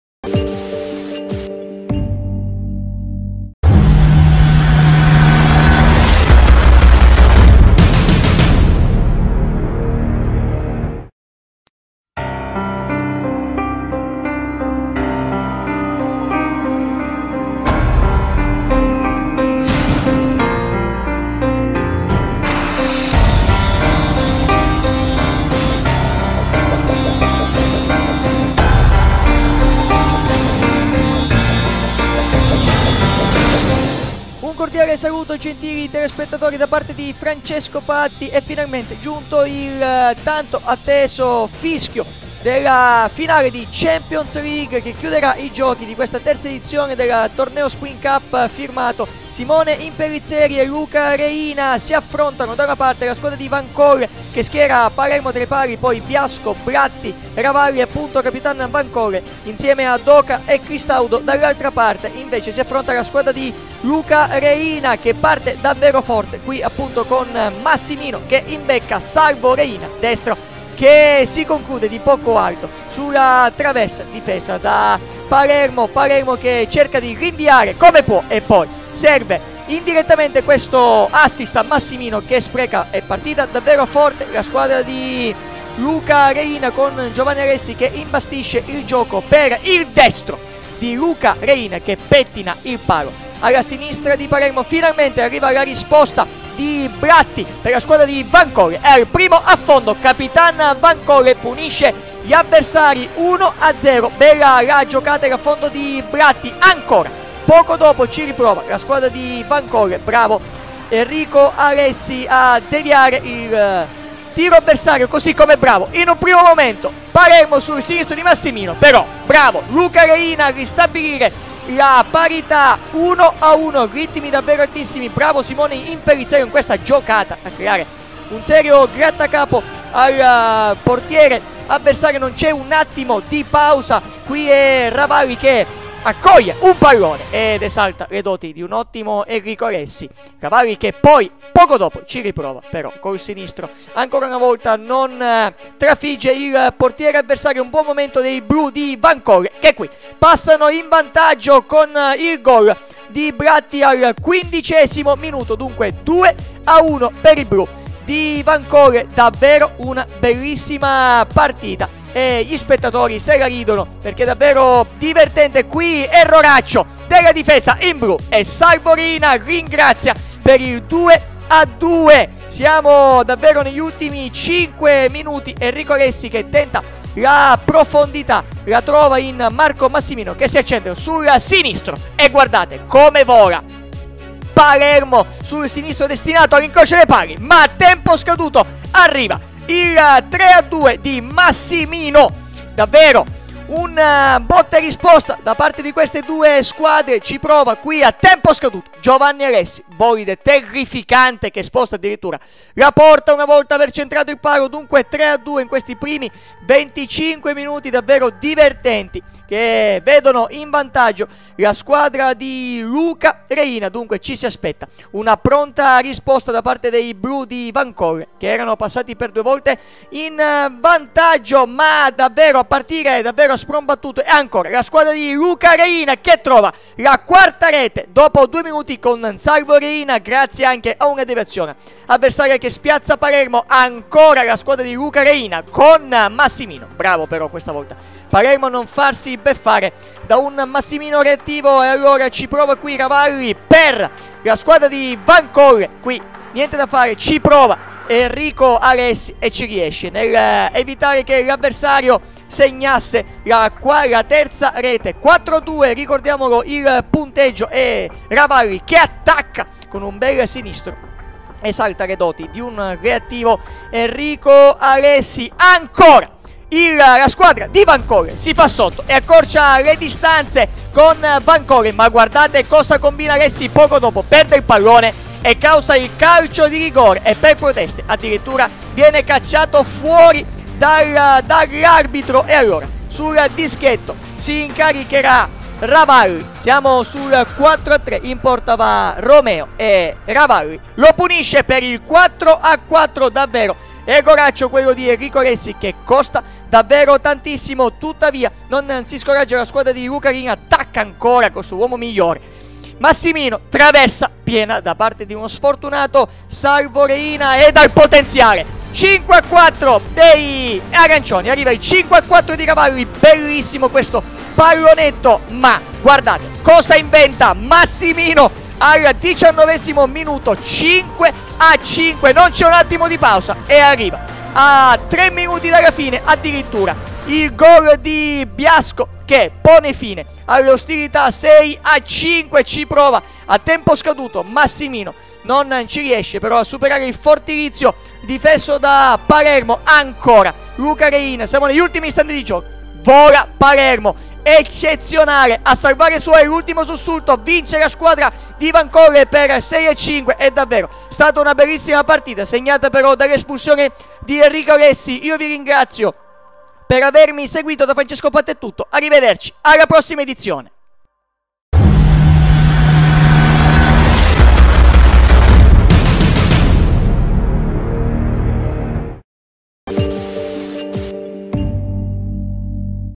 Finale Terza Ed. Spring Cup - Wancolle v Reina
Spring Cup

Organizzatori: Simone Impellizzeri, Luca Reina
Video e montaggio: Carlo Saitta
Commento tecnico: Francesco Patti